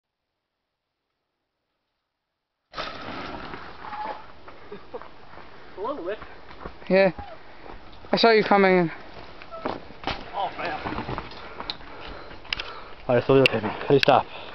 Mountain Bike Whip
He Goes off a jump and does a mountain bike whip